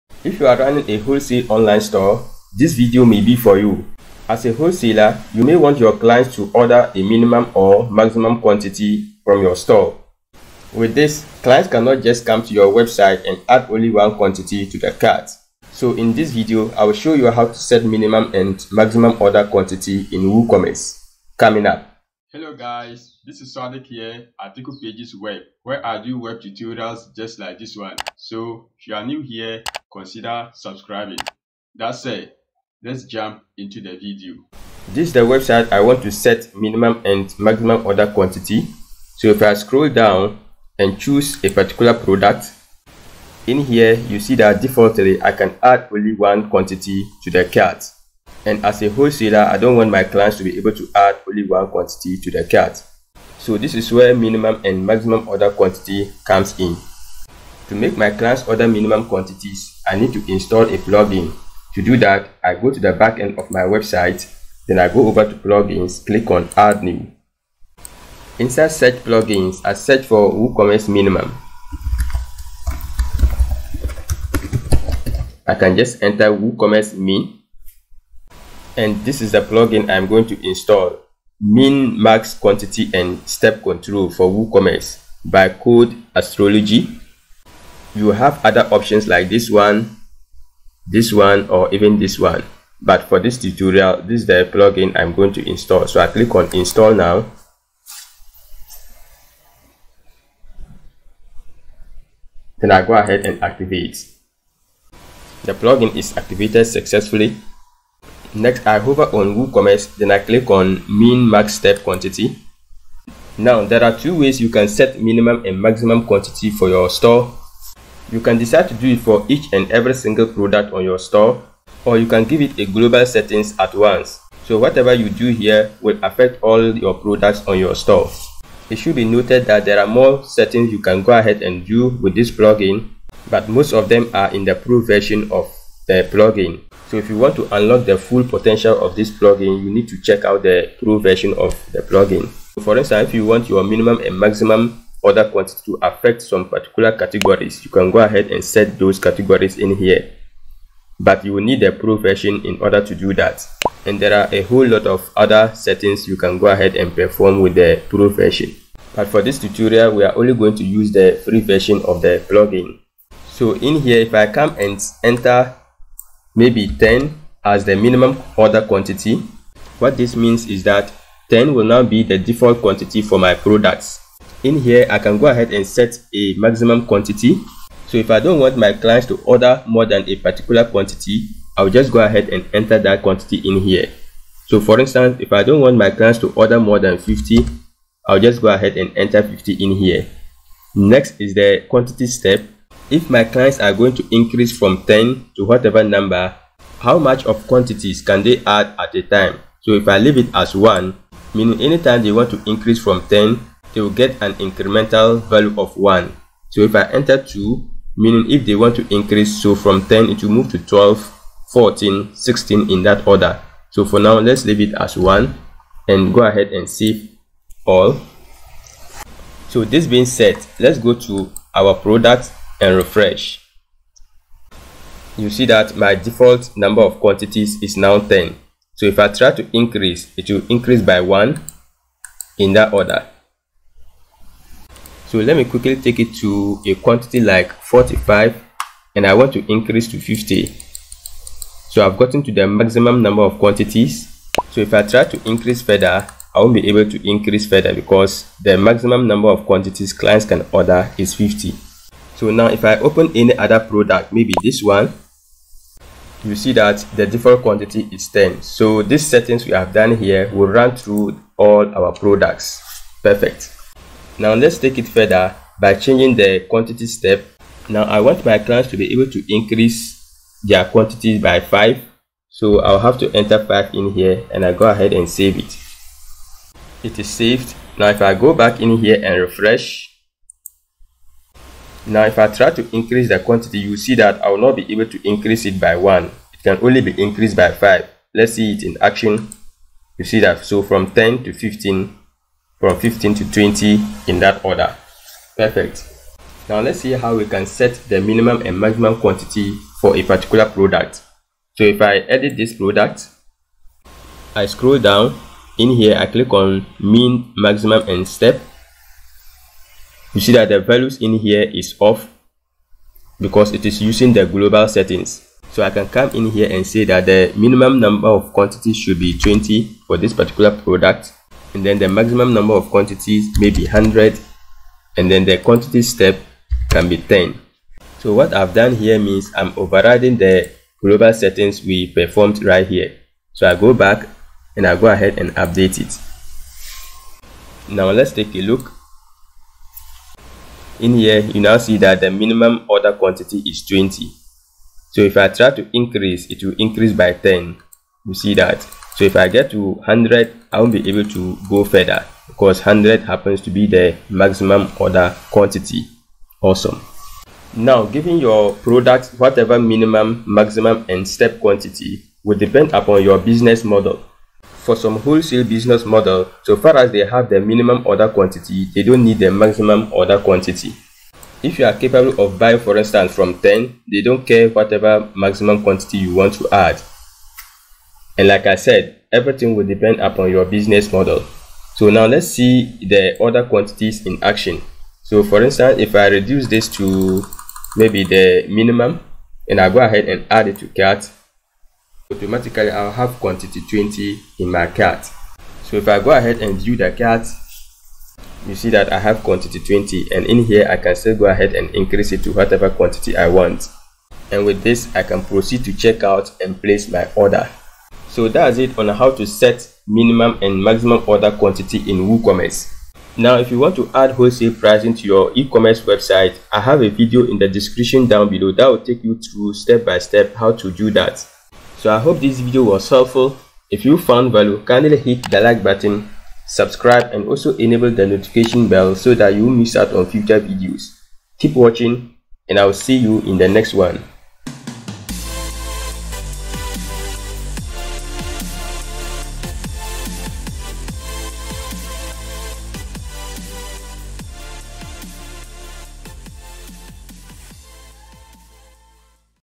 How to Set Minimum and Maximum Order Quantity in WooCommerce
In this woocommerce tutorial, I'll show you how to set minimum and maximum order quantity in woocommerce. This video is for you if you're a wholesaler and running an online store. As a wholesaler, you may want your clients to order a minimum or maximum from your store. With this clients cannot just come to your store and add only one quantity to the cart.

✅ HIRE ME: ✅
Please hire me for your WordPress, Elementor, and WooCommerce projects at: